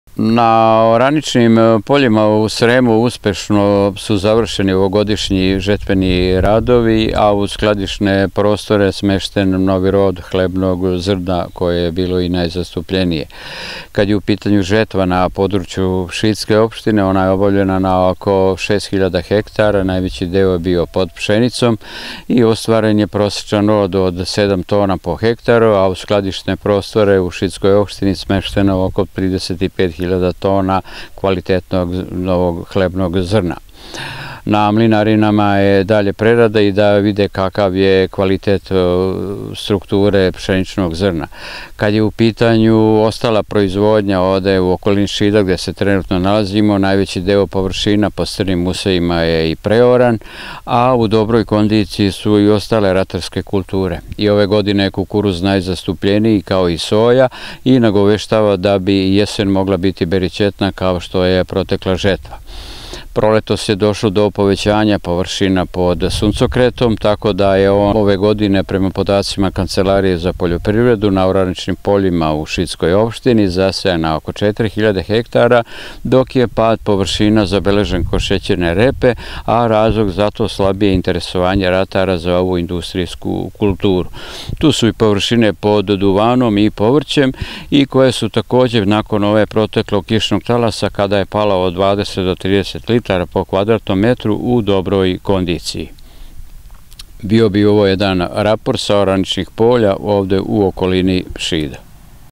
Stanje useva
Sa oko 5 hiljada hektara ove godine skinut je rod od oko 35000 tona hlebnog zrna i ostvaren je prosečan prinos od sedam tona po hektaru uz visoku hektolitarsku težinu i zadovoljavajući kvalitet, a najveći deo povšina je pooran i spreman za novu proizvodnju. Stanje ostalih ratarskih useva je zadovoljavajuće